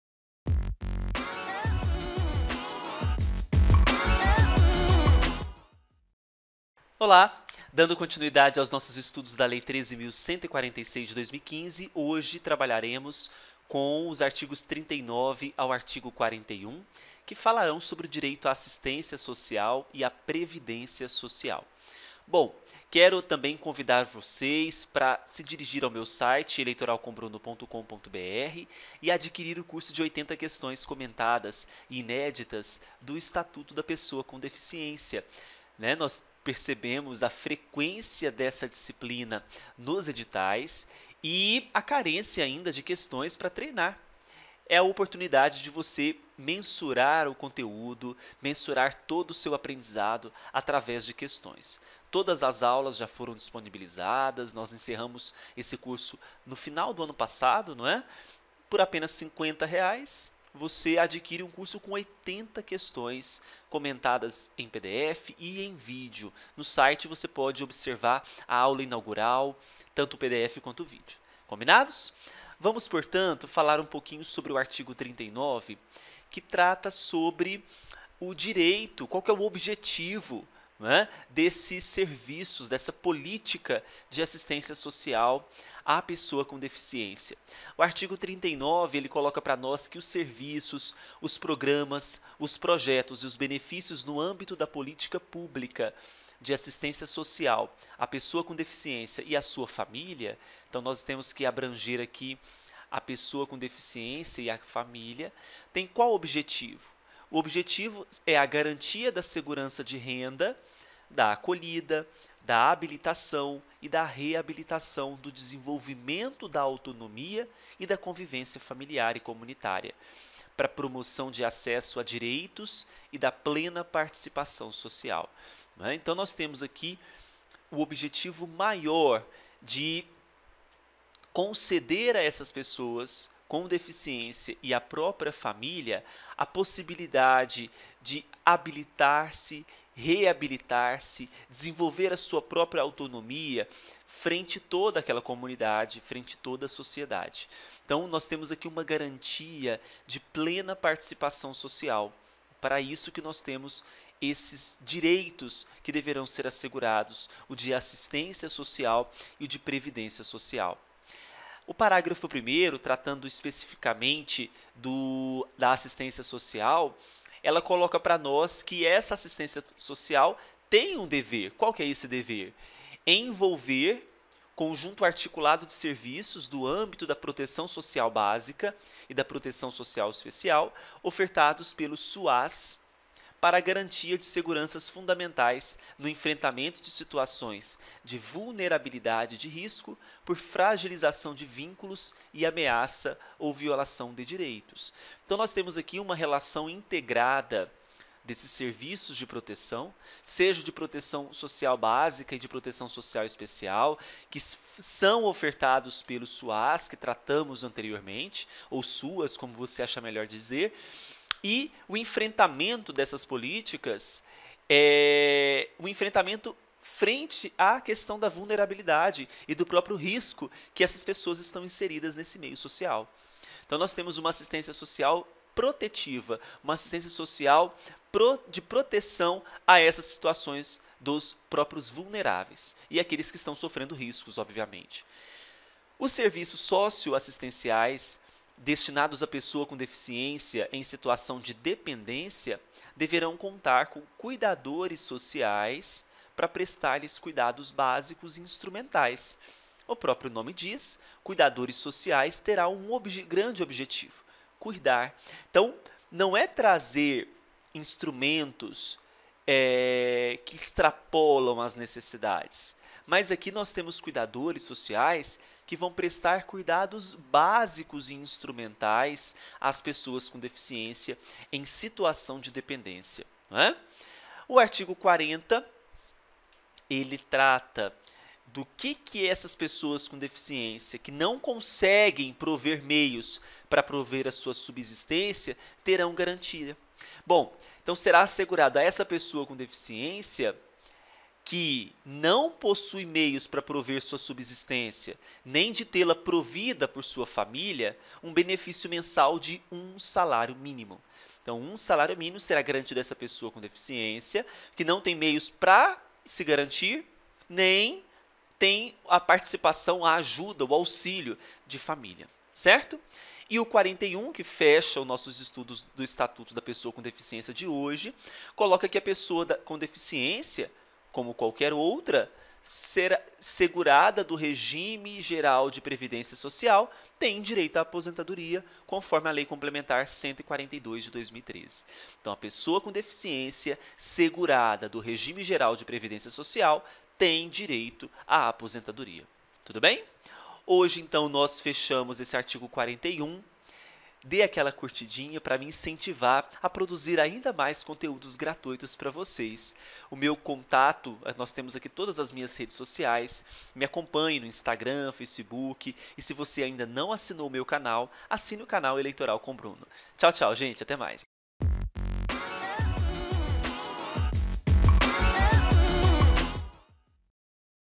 Estatuto da Pessoa com Deficiência - Art. 39 ao 41 - Direito à Assistência e Previdência Social
ATENÇÃO ! Esse curso foi gravado em 2018, portanto houve algumas alterações na lei. Por isso, sugiro o acompanhamento das aulas em conjunto com a lei seca. Um grande abraço! Professor Bruno!!!